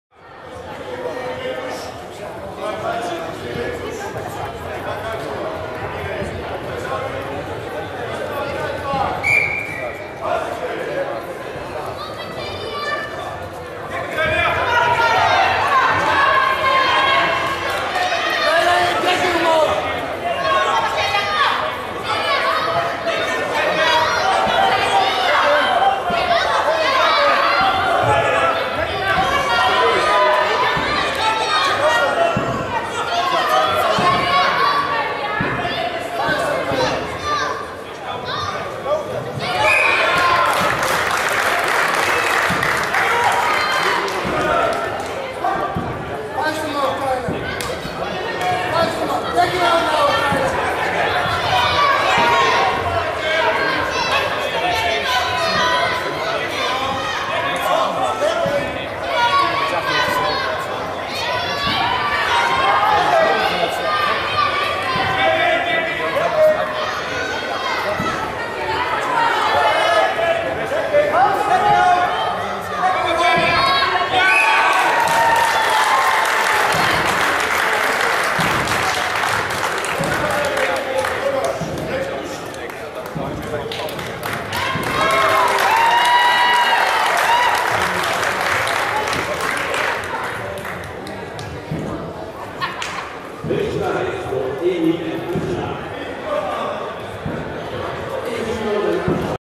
Kaylen Dieke vs Acelya Kulac
Karate Kaylen Dieke vs Acelya Kulac @ Full Contact Karate Matchmaking Tournament Groninger Slag 2014, Sporthal Selwerd Groningen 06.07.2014.